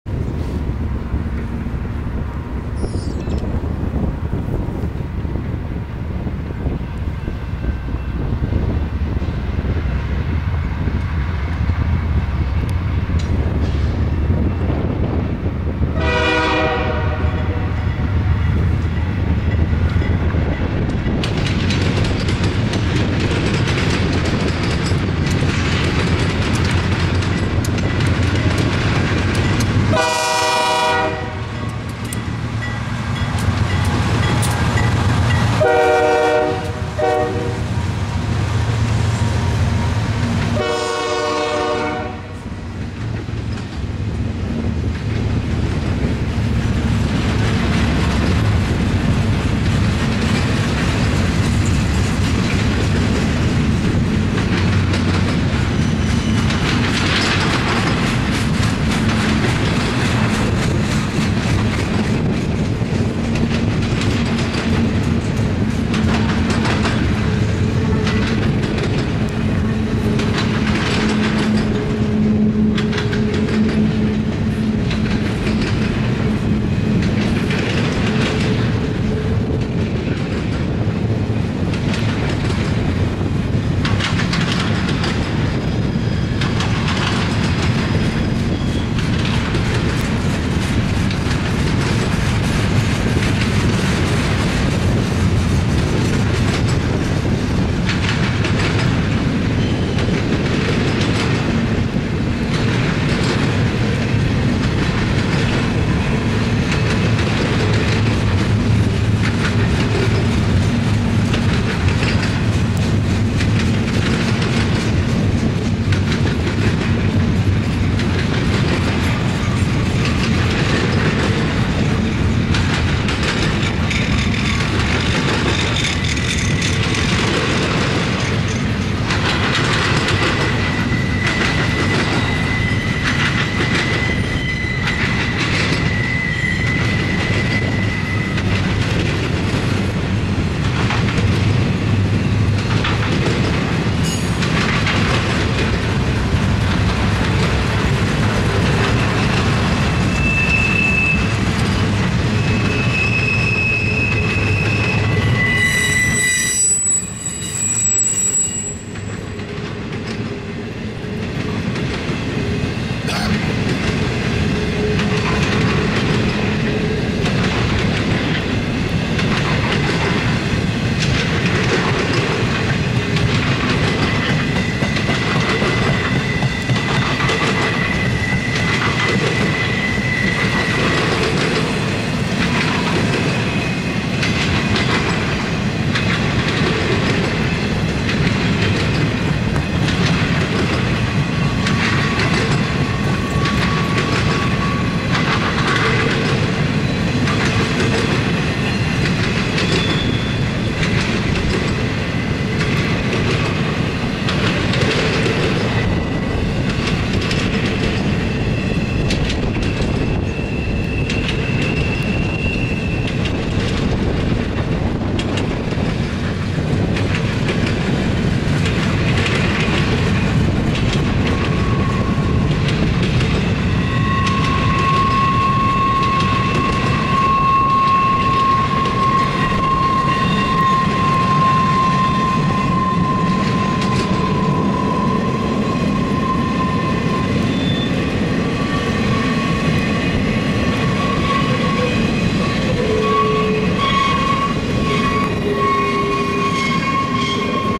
CSXT 7313, 251 & 5465 Leads an Intermodal East, Dolton, IL
CSX Barr Subdivision
Filmed 1/24/10